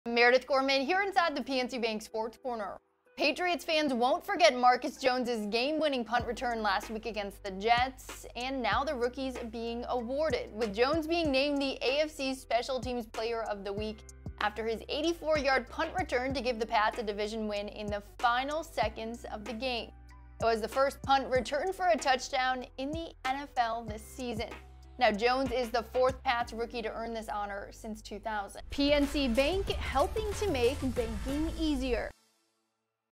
Patriots Rookie Marcus Jones Named AFC Special Teams Player Of The Week
Patriots rookie Marcus Jones was awarded the AFC Special Teams Player of the Week for his thrilling 84-yard punt return touchdown in their win over the Jets.